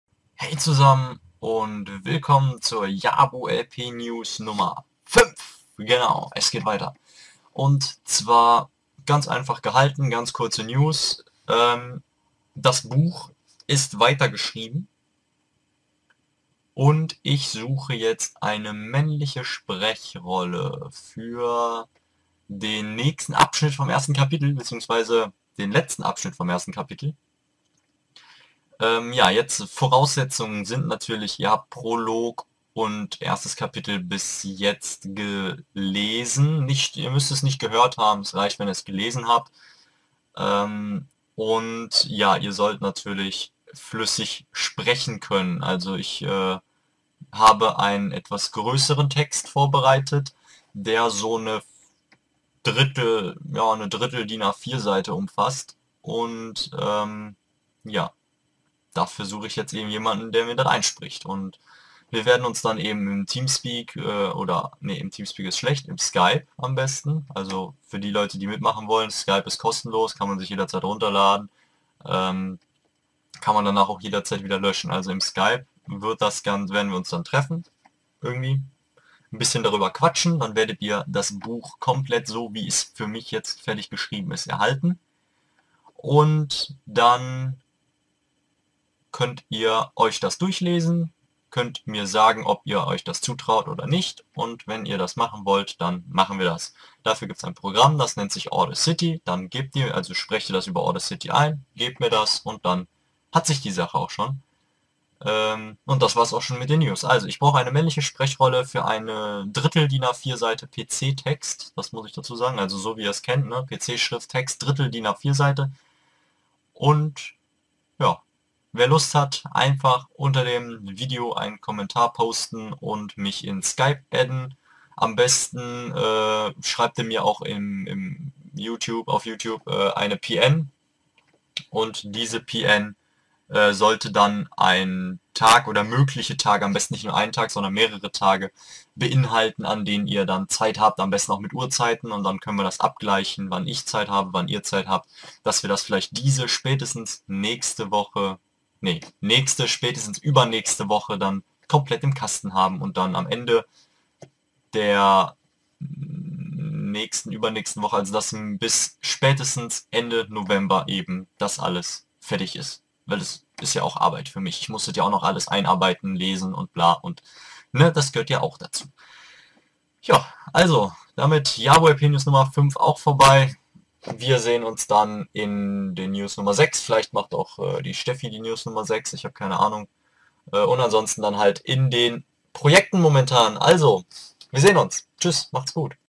JabuhLP News #05 - männliche Sprechrolle gesucht
Die fünfte Ausgabe unserer Kanalnews. Immer wenn wir Neuigkeiten für euch parat haben werden wir diese in den JabuhLP News preisgeben =) Also seid auf der Hut vor unseren News ;D